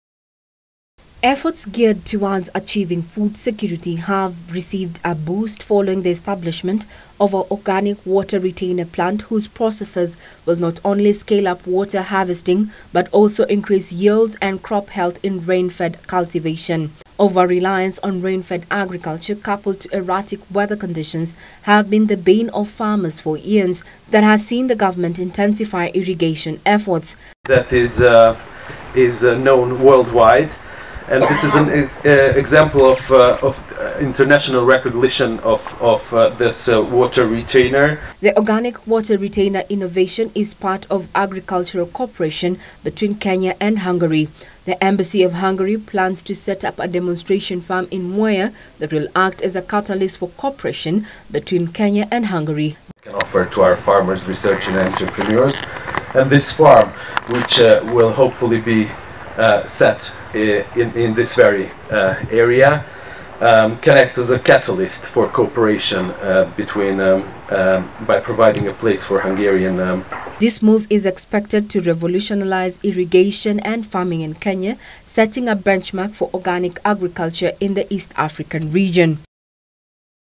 Hungarian gov't establishes organic Water Retainer solution plant in Kirinyaga
The Hungarian government has established an organic Water Retainer solution plant in Kirinyaga County whose processes are aimed at enhancing water storage as well as increases yield and crop health in rain-fed cultivation in the larger Mt. Kenya region. Experts say the technology will also reduce cases of soil degradation as well as regulate soil water balance ensuring endurance of crops during dry weather patterns.